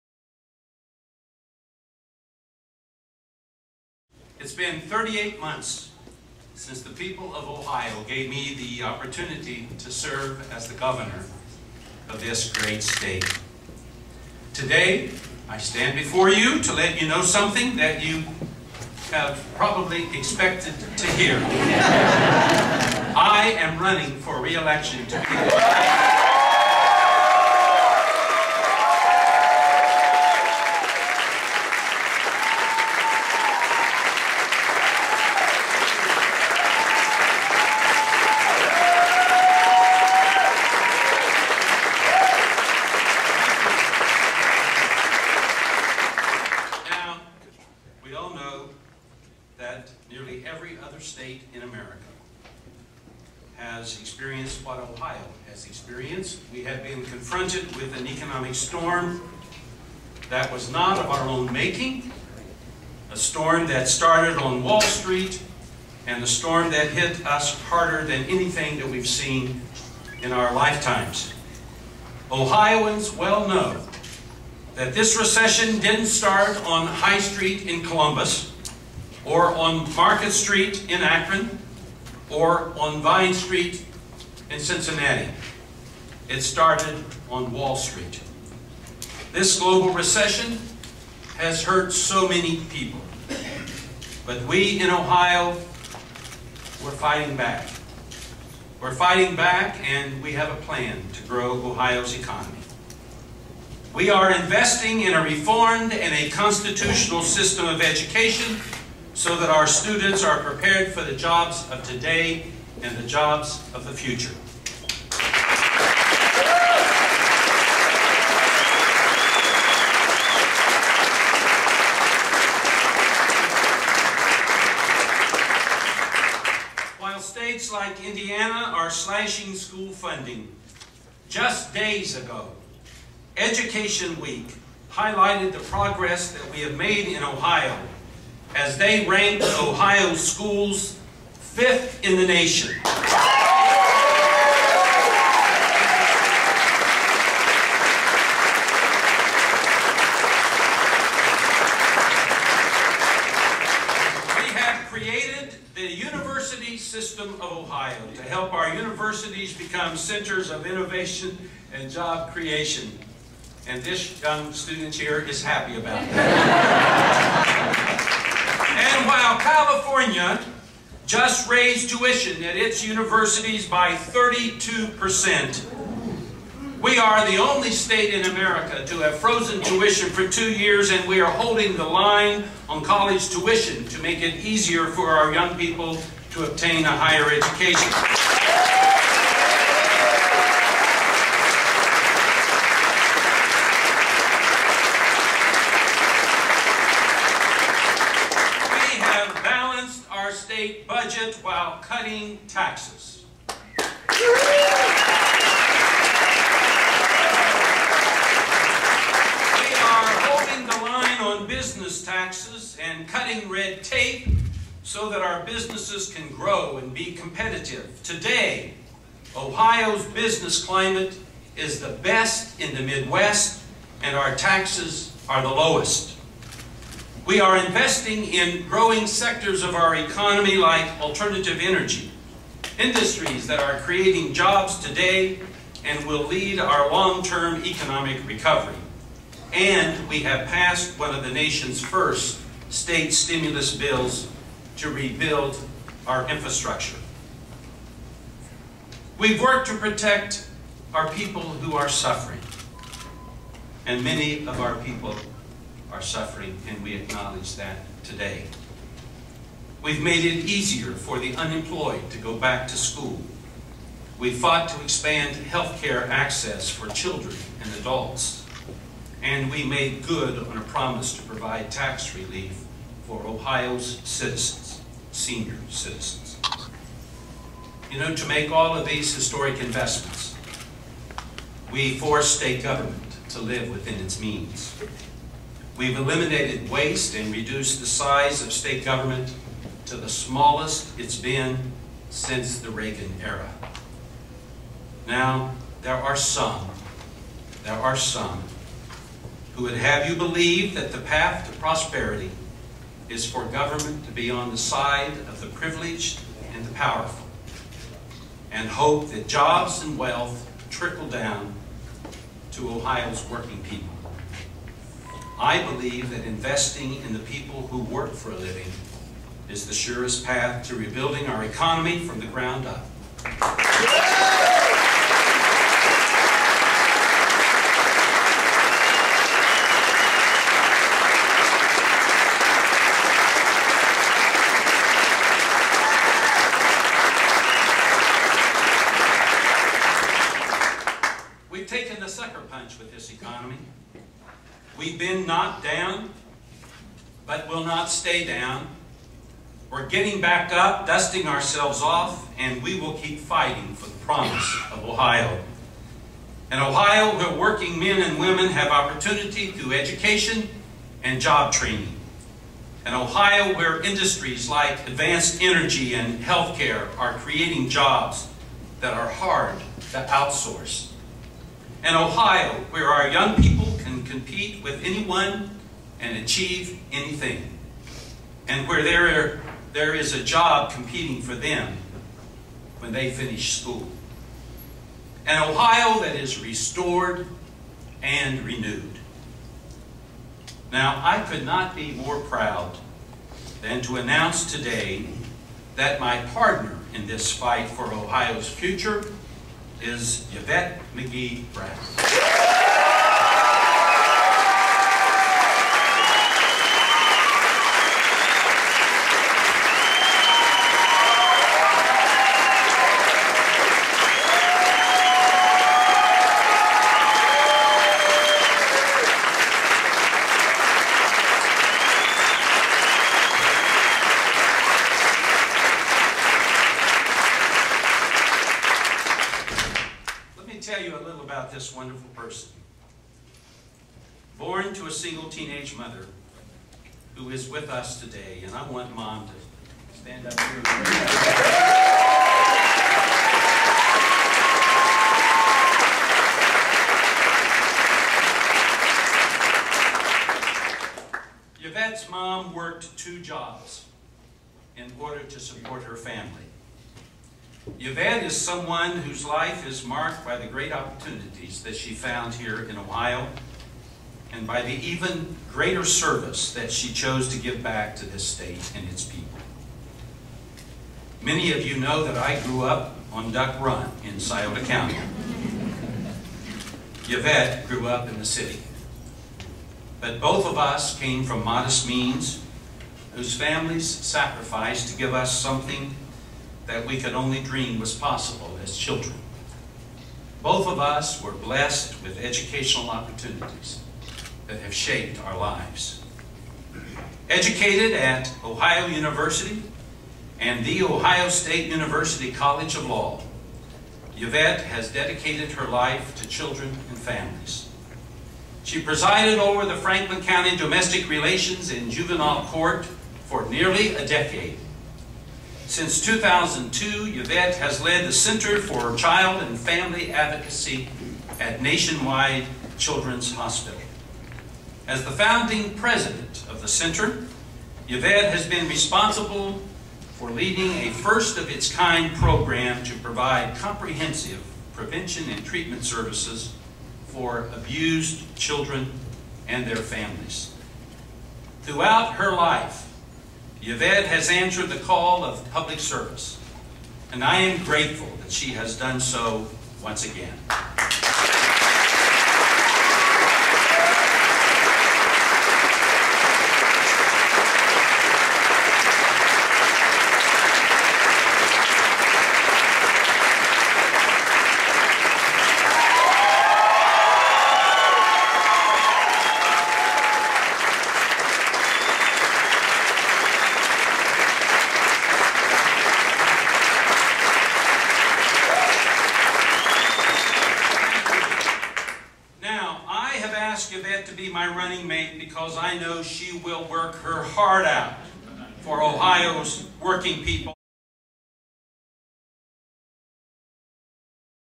Governor Ted Strickland Announces Yvette McGee Brown As Running Mate
COLUMBUS--At a rally announcing his bid for re-election, Governor Ted Strickland today introduced his running mate and the Democratic candidate for Lieutenant Governor, Yvette McGee Brown. Highlighting the need to lay the foundation for Ohio's long-term economic growth, Strickland emphasized that McGee Brown would be a critical partner in fighting to expand opportunity and create an economy that works for Ohio families.